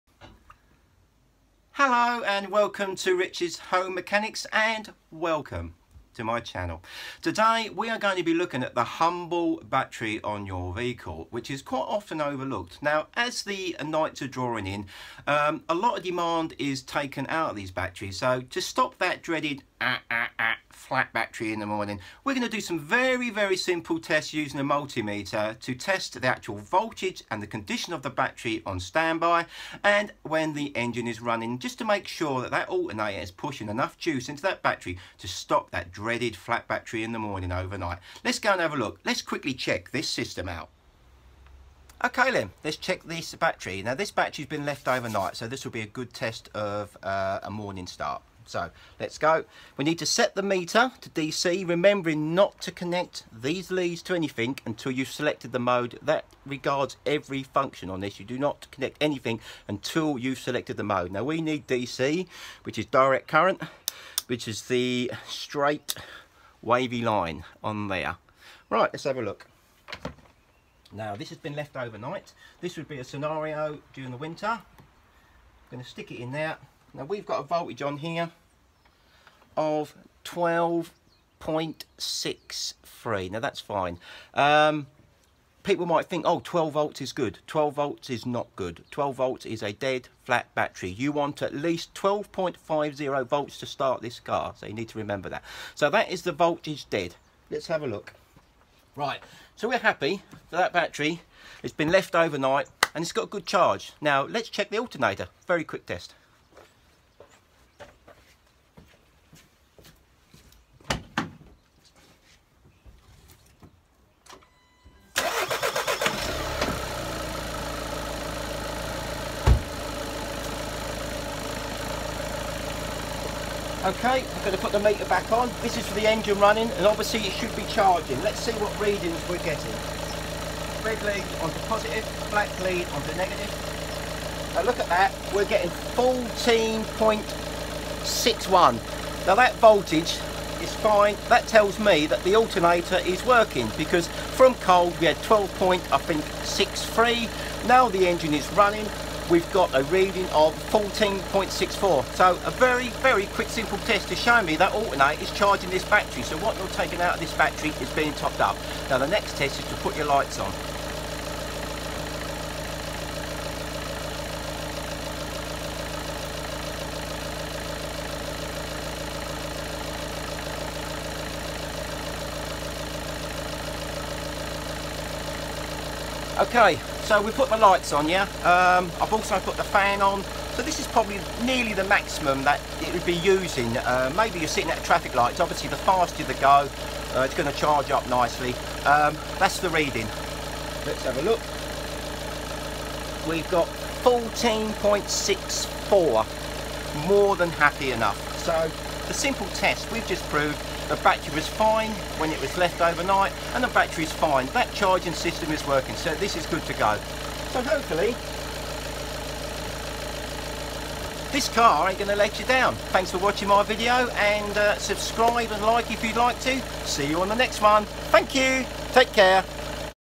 How to check a car battery
How to test a car battery when the vehicle has been left over night and how to test that the alternator is charging the battery